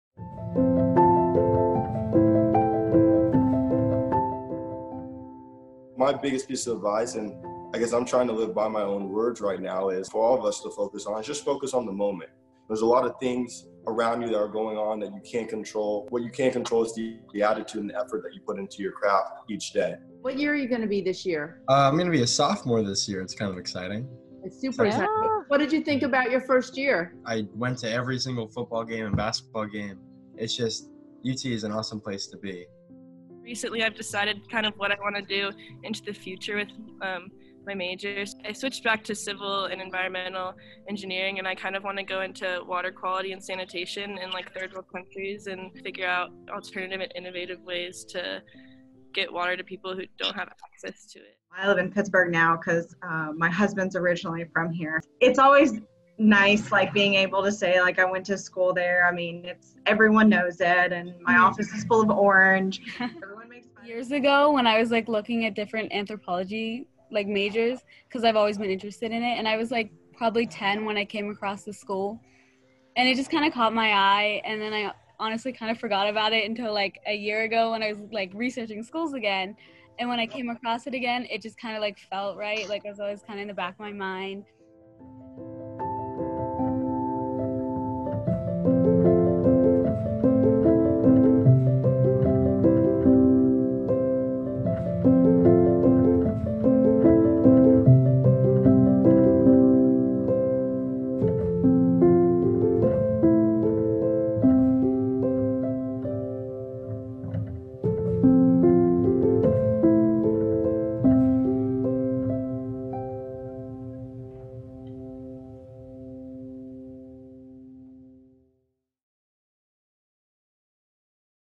UT Alumni Chapters Award Scholarships Nationwide
Forty-four official UT alumni chapters exist across the nation, and while all offer some financial assistance to students from their areas attending UT, many offer endowed scholarships.